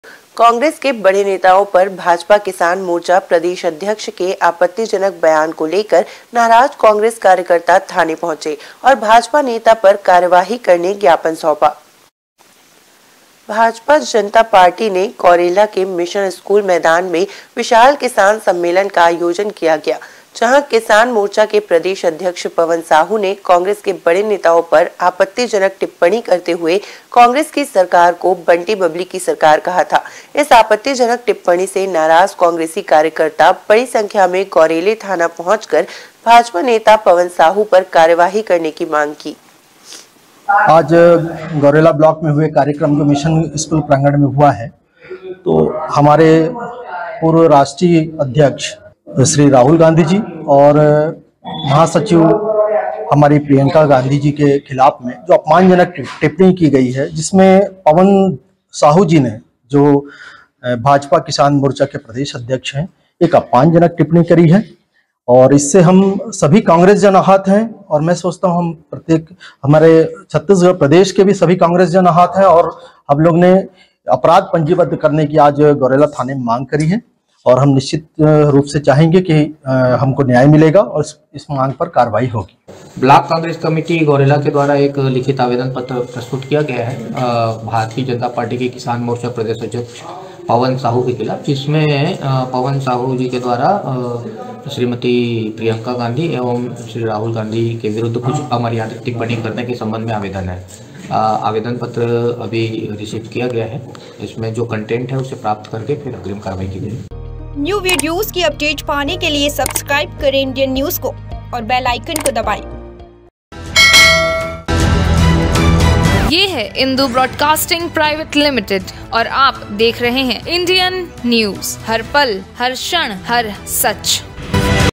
विवादित टिप्पणी से कांग्रेसियों में नाराजगी, गौरेला थाने पहुंच कांग्रेसियों ने सौंपा ज्ञापन
Indian News : कांग्रेस के बड़े नेताओं पर भाजपा किसान मोर्चा प्रदेश अध्यक्ष के आपत्तिजनक बयान को लेकर नाराज कांग्रेस कार्यकर्ता थाने पहुंचे और भाजपा नेता पर कार्यवाही करने ज्ञापन सौंपा।

Our Links